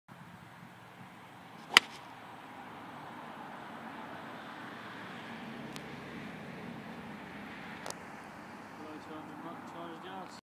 colin marsh pga tour furzeley
colin marsh hits a absolute beauty at the pga tour furzeley hole 5 par 4. this man is a magician!